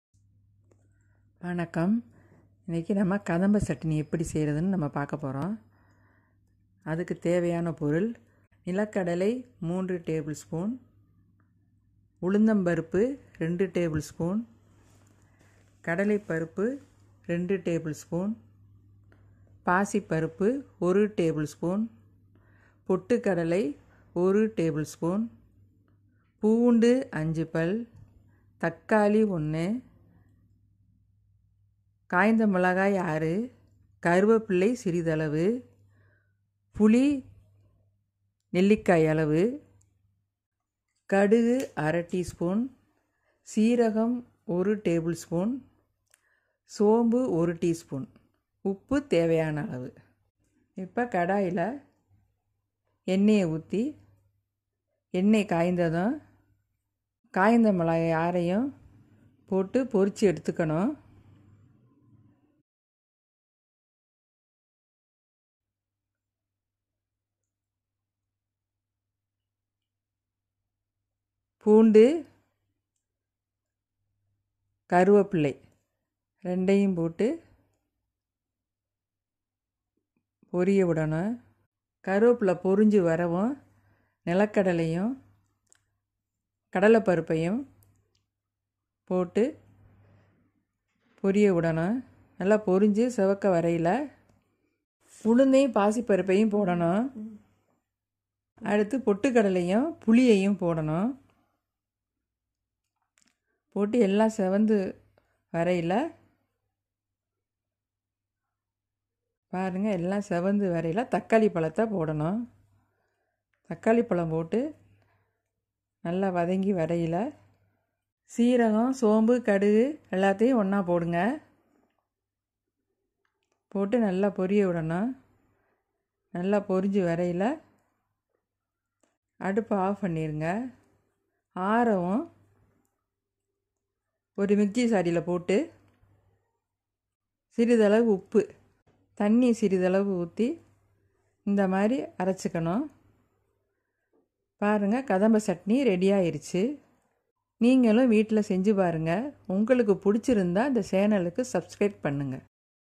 Kadamba Chutney
ingredient
3tbsp peanut
2tbsp urud daal
2tbsp channa daal
1tbsp moong daal
1tbsp roasted gram
5 gloves garlic
1 tomato
6 dry chili
Curry leaves
1 lime size tamarind
1/2tsp mustard seeds
1tbsp cumin seeds
1tsp fennal seeds
Salt

Procedure
1. In a kadai, heat up oil, fry dry chili and keep it aside for later use
2. In the same oil, add in garlic, curry leave and saute until golden brown
3. Add in peanut and channa daal fry until peanut turn crisp
4. Add in urud daal  and Moong daal
5. Add in tamarind and roasted gram and mix well
6. Add in tomato and fry till tomato gets soft
7. Add in fennel seeds, cumin seeds and mustard seeds and continue frying for few minutes and turn off the flame
8. Blend the mixture into smooth paste after the mixture cools down